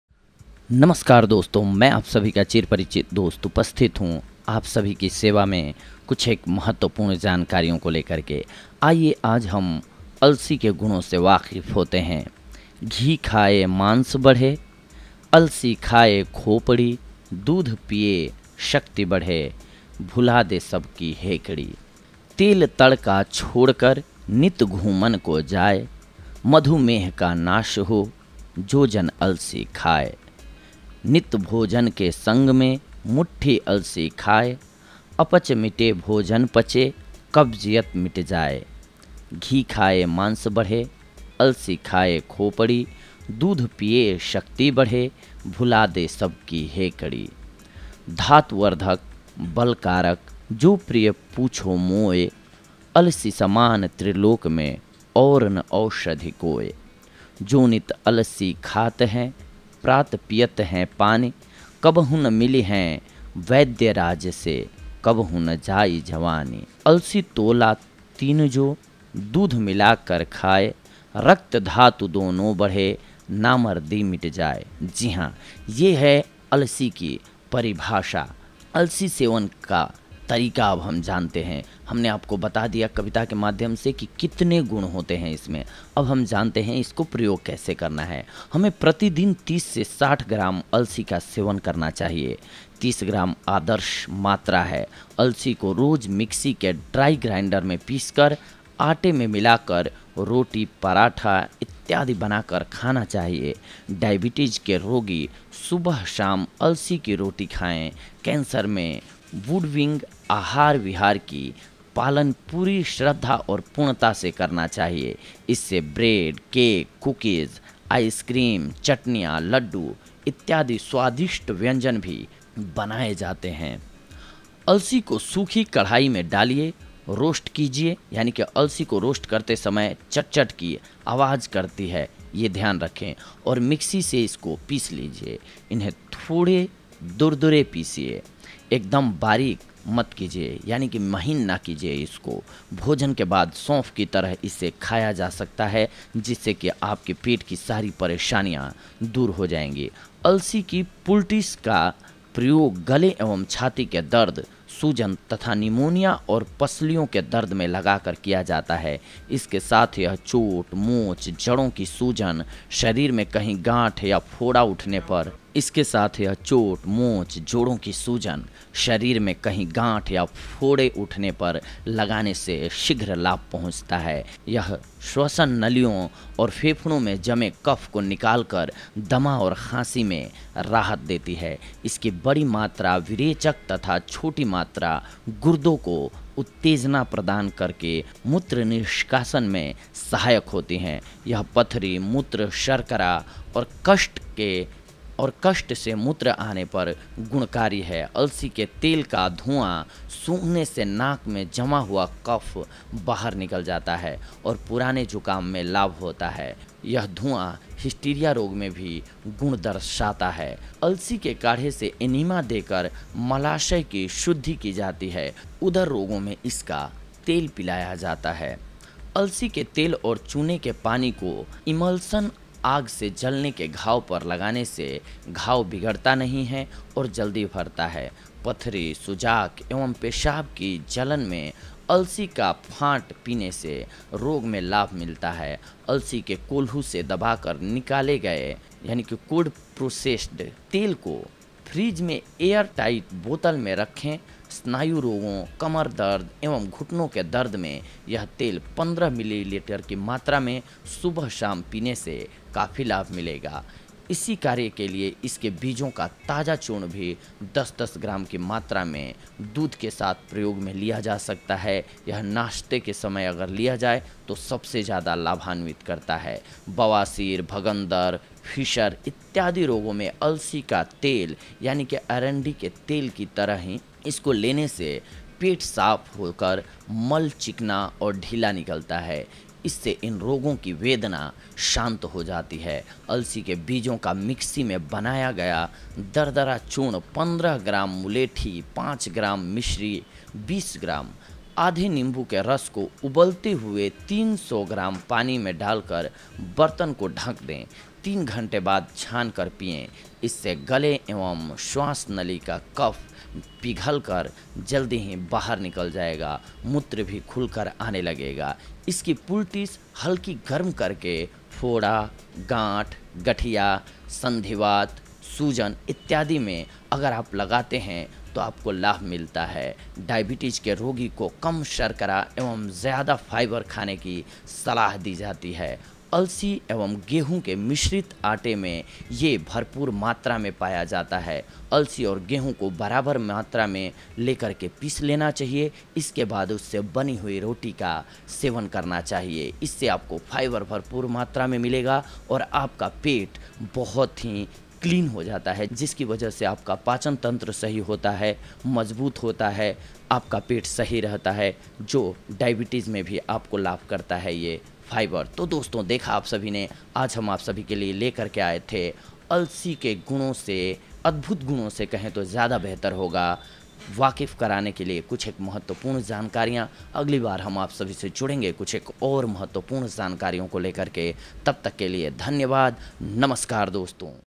1 चम्मच ये बीज अलसी खा ले 90 साल तक नहीं होगा घुटनों और जोड़ों में दर्द। रामबाण nuskha
120साल कर जीने की गारंटी देती है अलसी ,लेकिन तरीका ये होना चाहिये//